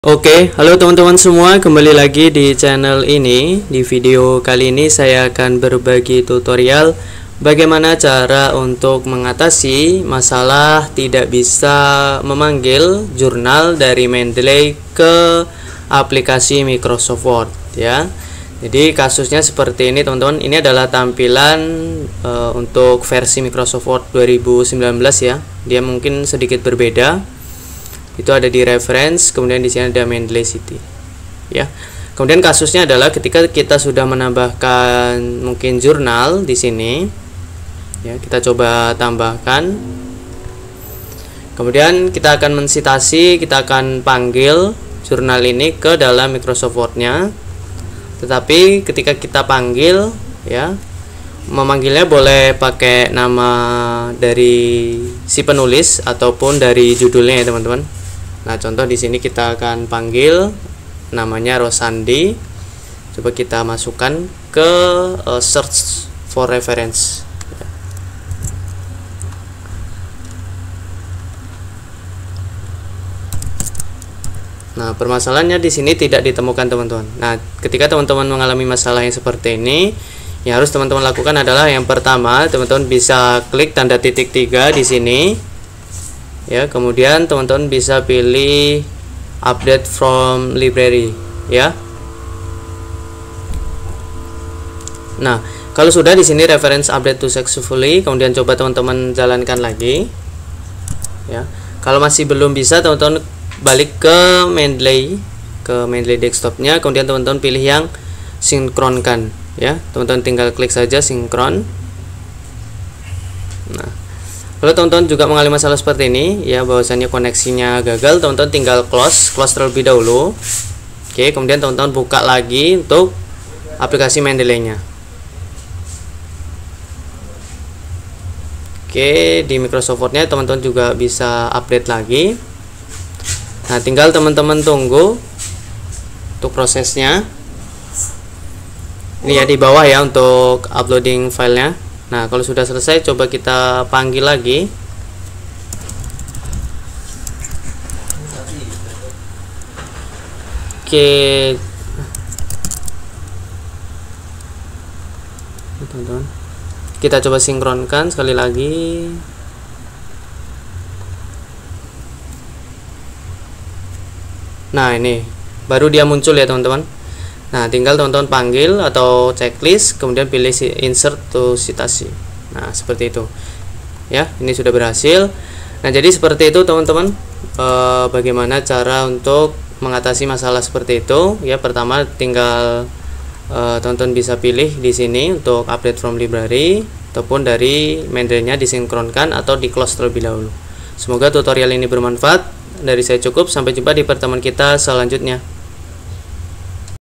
Cara Mengatasi Jurnal Mendeley Tidak Terbaca di Microsoft Word
halo sahabatku kembali lagi di chanel ini kali ini saya mau berbagi tutorial bagaimana Cara Mengatasi Jurnal Mendeley Tidak Terbaca di Microsoft Word

Dukungan dari kalian sangat bermanfaat bagi saya